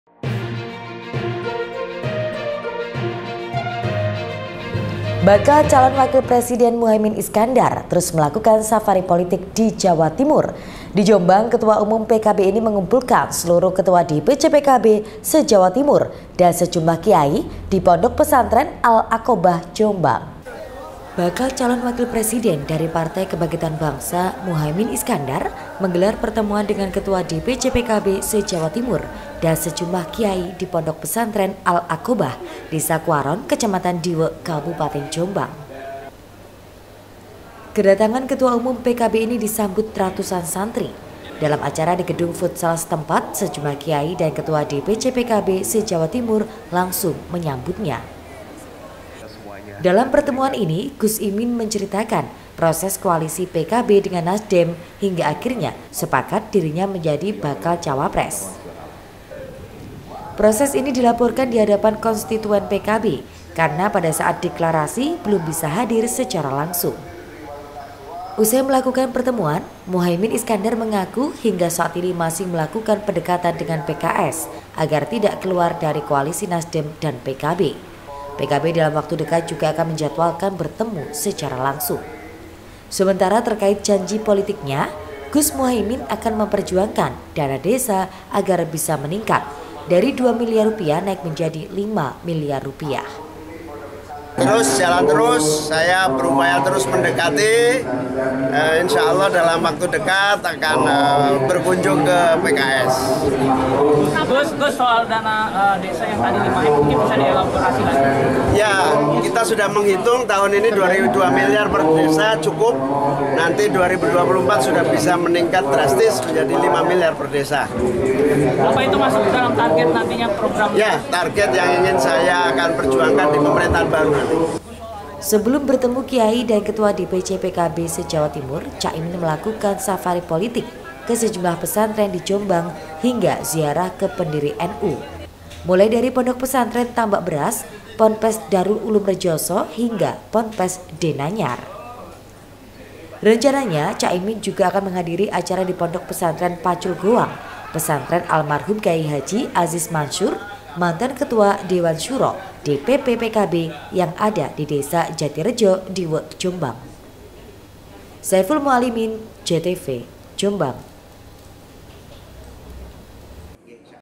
Safari di Jatim, Cak Imin Kumpulkan Ketua DPC PKB & Kiai Ponpes
JOMBANG - Bakal calon wakil presiden muhaimin iskandar, terus melakukan safari politik di jawa timur. Di jombang, ketua umum pkb ini mengumpulkan seluruh ketua dpc pkb se jawa timur dan sejumlah kiai di pondok pesantren al aqobah jombang.